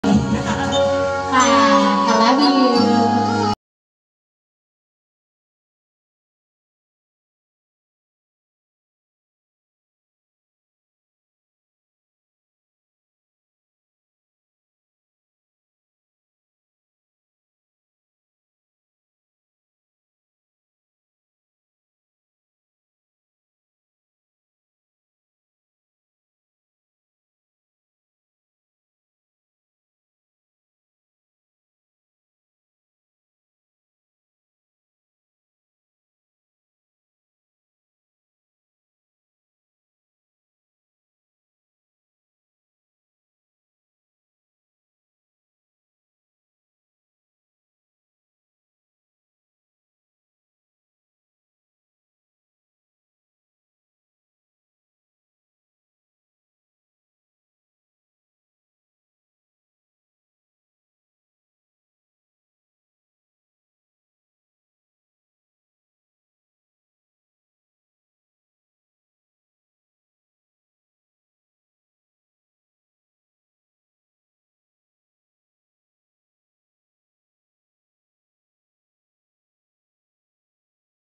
𝐩𝐢𝐧𝐚𝐠𝐢𝐬𝐢𝐧𝐠 𝐚𝐤𝐨👀 𝟏𝟎𝐩𝐦 𝐧𝐚 𝐚𝐭𝐚 𝐭𝐨𝐡 𝐞𝐡, 𝐩𝐚𝐫𝐚 𝐥𝐚𝐧𝐠 𝐤𝐚𝐧𝐭𝐚𝐡𝐚𝐧 𝐊𝐎 𝐝𝐚𝐰 𝐬𝐲𝐚🤣 𝐬𝐢 𝐩𝐚𝐩𝐚 𝐭𝐚𝐥𝐚𝐠𝐚😘
𝒕𝒉𝒂𝒏𝒌 𝒚𝒐𝒖 𝒔𝒂 𝒏𝒂𝒈𝒗𝒊𝒅𝒆𝒐 𝒏𝒂 𝒌𝒂𝒑𝒊𝒕𝒃𝒂𝒉𝒂𝒚 𝒌𝒐 👀 𝒔𝒖𝒃𝒓𝒂𝒏𝒈 𝒛𝒐𝒐𝒎 𝒂𝒕 𝒍𝒂𝒃𝒐 𝒉𝒂𝒉𝒂 𝒌𝒂𝒉𝒊𝒕 𝒔𝒉𝒐𝒓𝒕 𝒍𝒂𝒏𝒈 𝒏𝒂 𝒂𝒑𝒑𝒓𝒆𝒄𝒊𝒂𝒕𝒆 𝑲𝑶 ☺